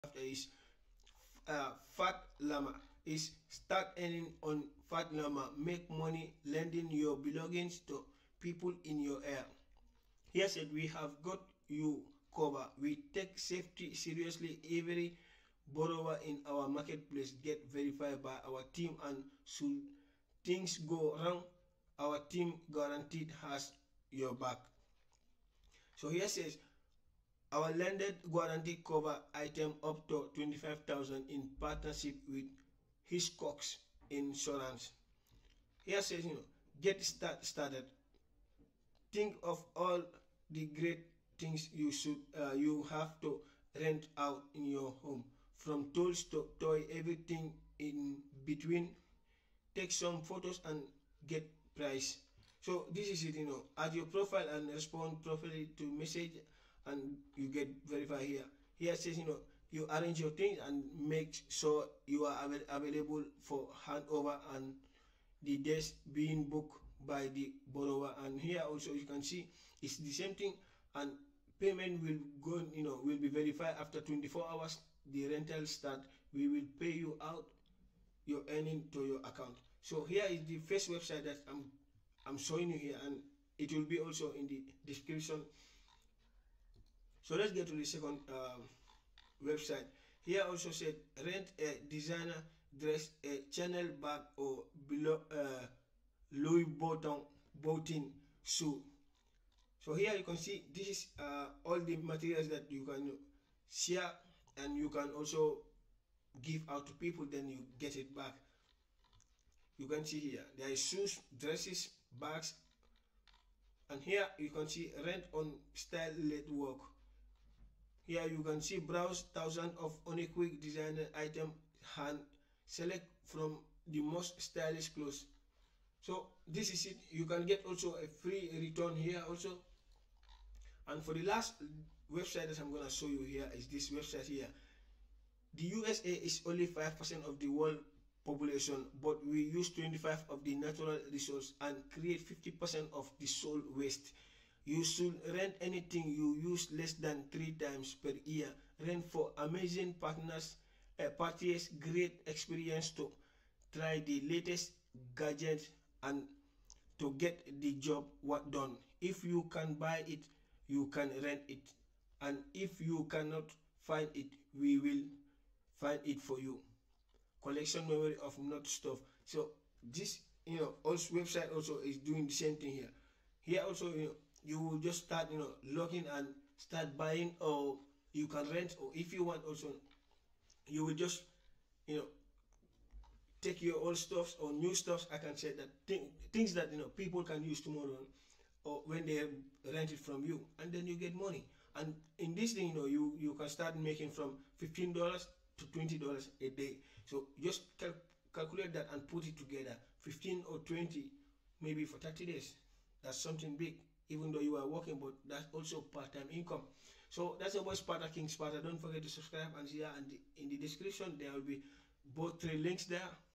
Rental as a servics earning potential makes around $15 $20 a day
hi in this video I will show u how to make money with  Rental as a servics earning potential  makes around $15 $20 a day it's very easy to start up

4K Sony Vlog Camera

4K Plus USB Capture Card,

M1 Lavalier Wireless Microphone with Dual-Charging

Advanced Video Lighting Kit

Mini Video Light with Cold Shoe Mount